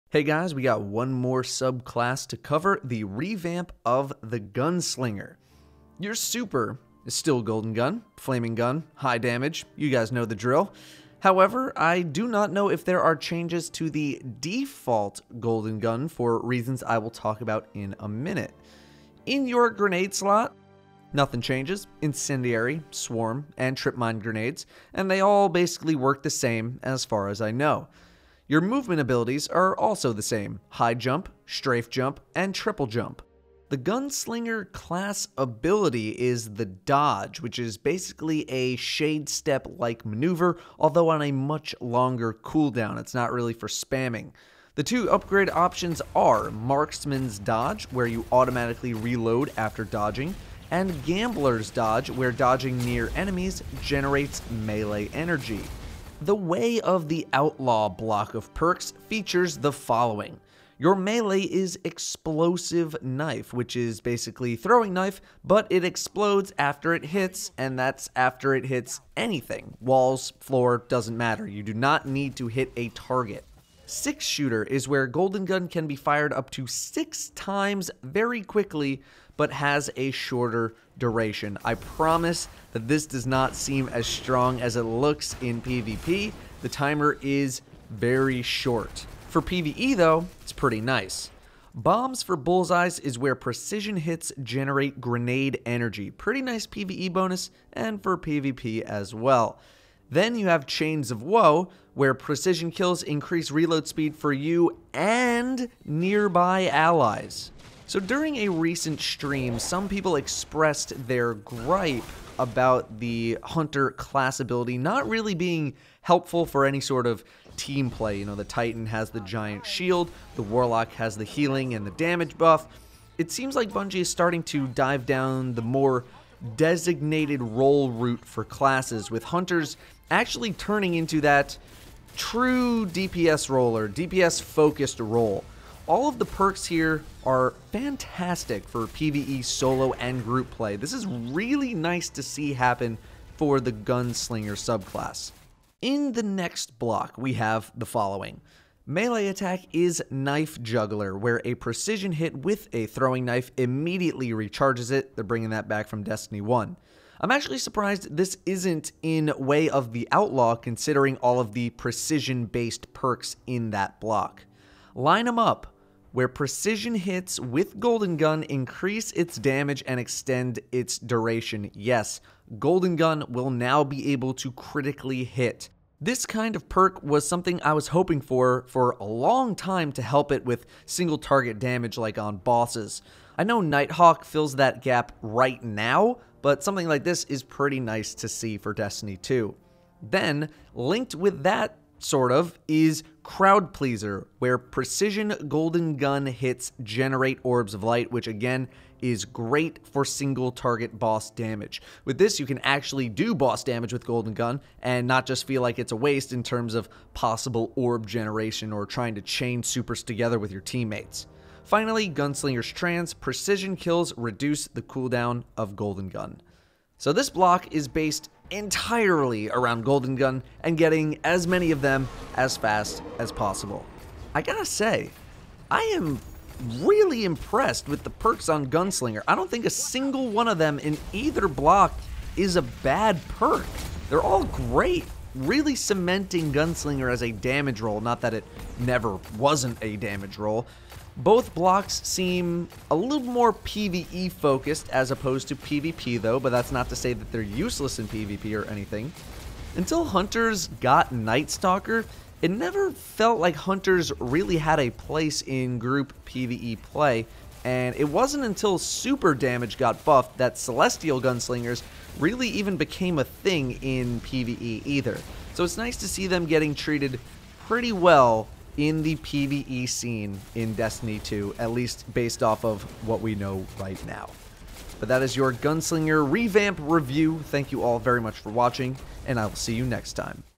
Destiny 2: The Gunslinger Hunter Subclass Revamp (New and Changed Perks)
In the final subclass review, we'll talk about why I think Gunslinger is actually going to be quite impressive in PvE.

(add CA for Canada, UK for United Kingdom, FR for France and DE for Germany)